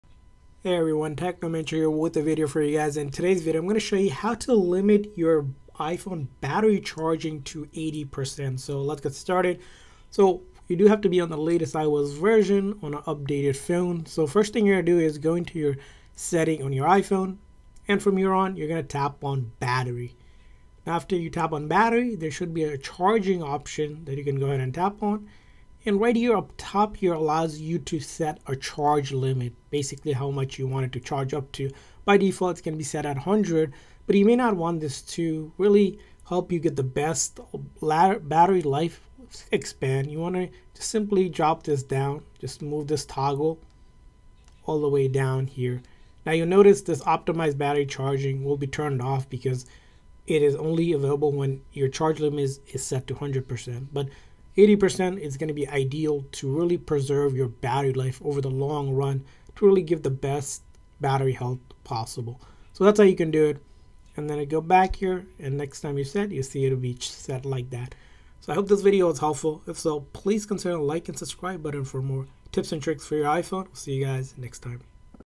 How To Limit iPhone Charging To 80%?
Here's how to limit iPhone battery charging to 80%!